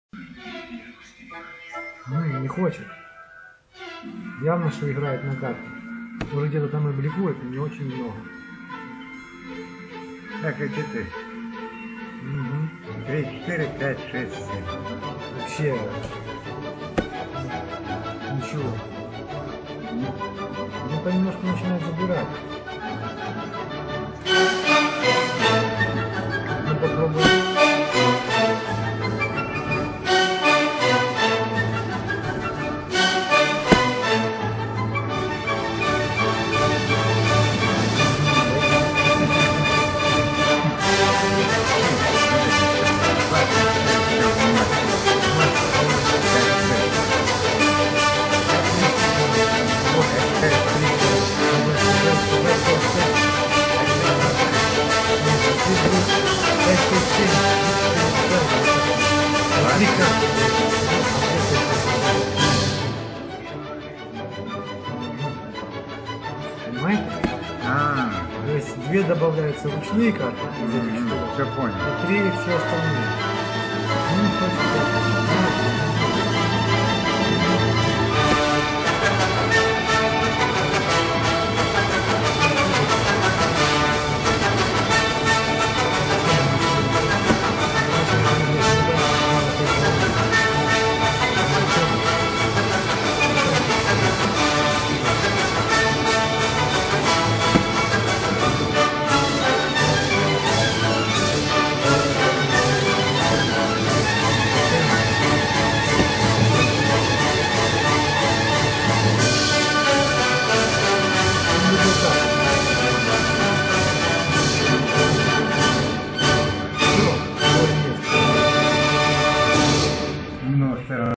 Как не надо играть ХА
Вообщем затупил. Надо было раньше ходить оллин. Но второе место тоже неплохо )))